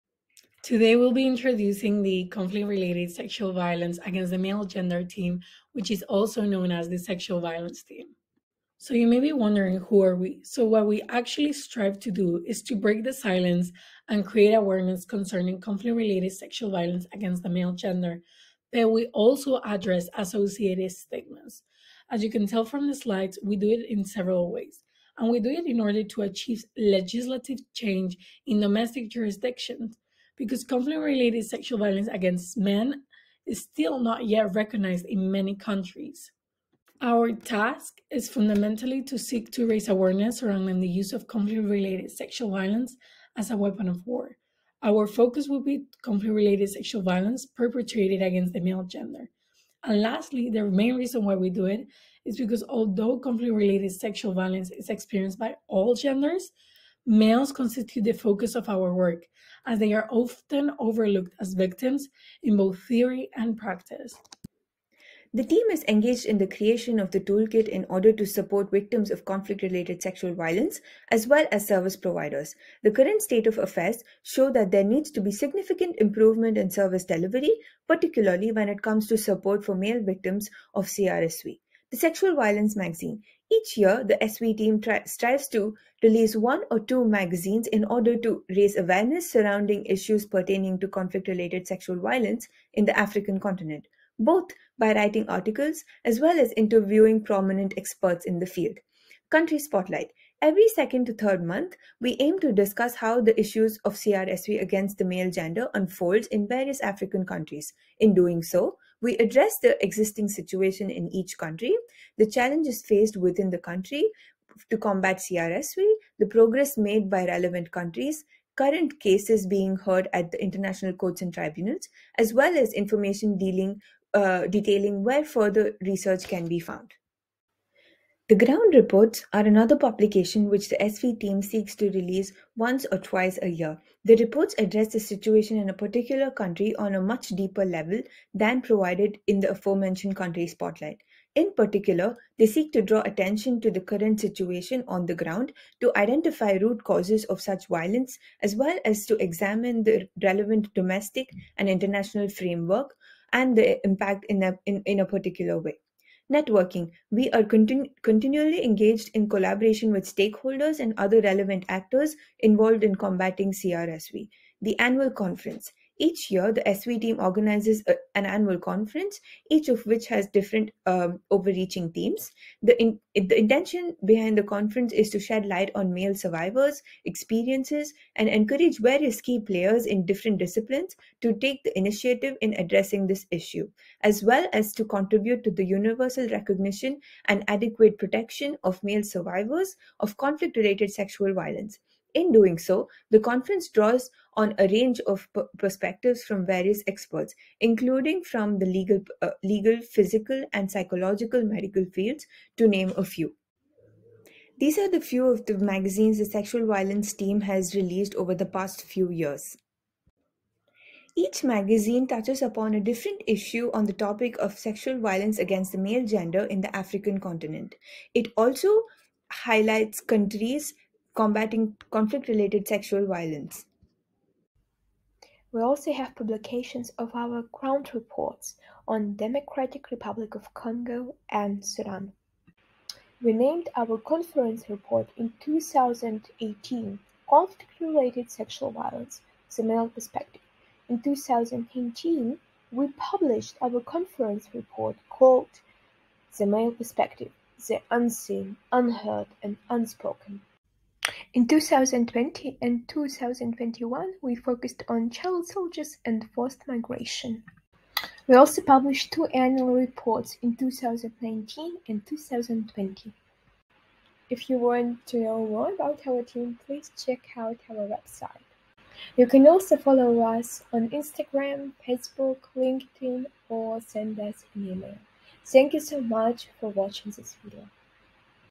Introducing the Conflict-Related Sexual Violence Against the Male Gender Team of the CAJPHR
The Centre for African Justice, Peace and Human Rights has 4 amazing teams that carry out its projects and activities. Here is a glimpse at one of these teams and the work they do.